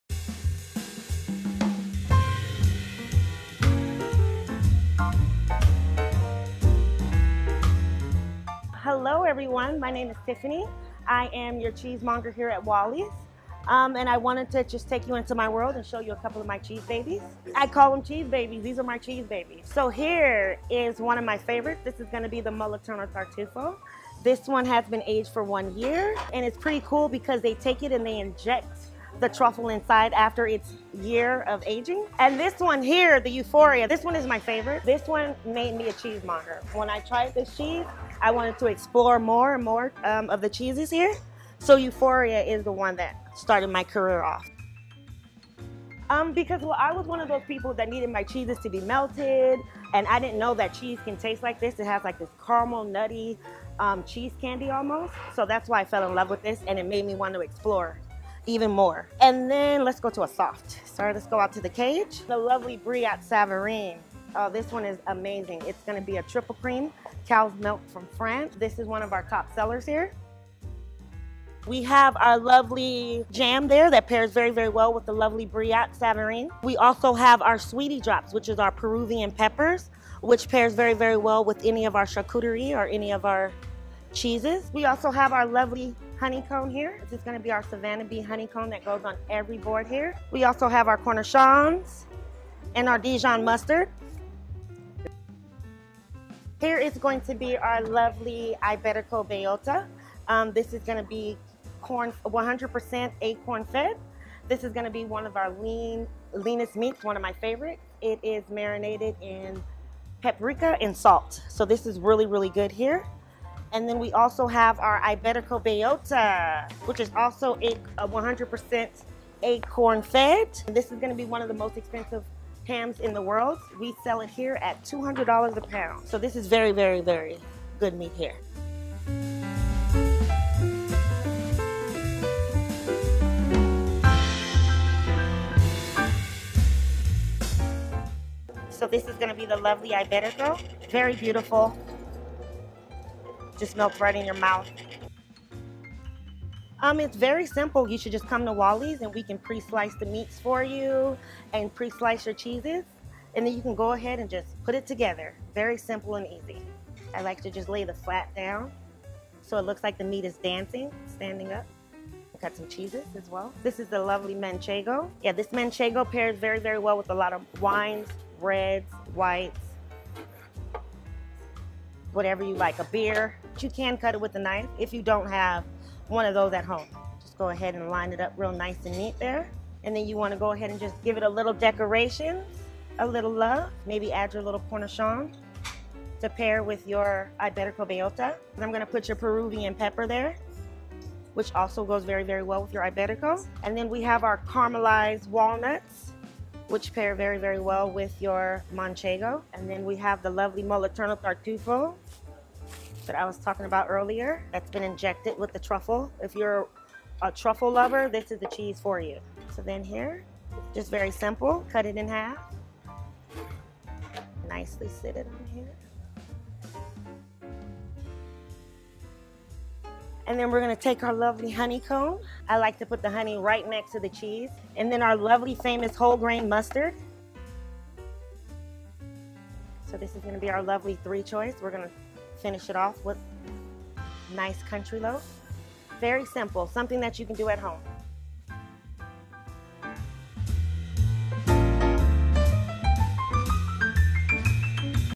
Charcuterie Board DIY with Cheesemonger Tiffany | Explore Wally's Gourmet Cheese Shop
Discover the art of charcuterie board crafting with Cheesemonger Tiffany at Wally's Gourmet Cheese Shop! Dive into a world of exquisite cheeses and gourmet delights as Tiffany shares insider tips and tricks as she demonstrates how to create a stunning charcuterie board at home, using our finest cheeses and accompaniments.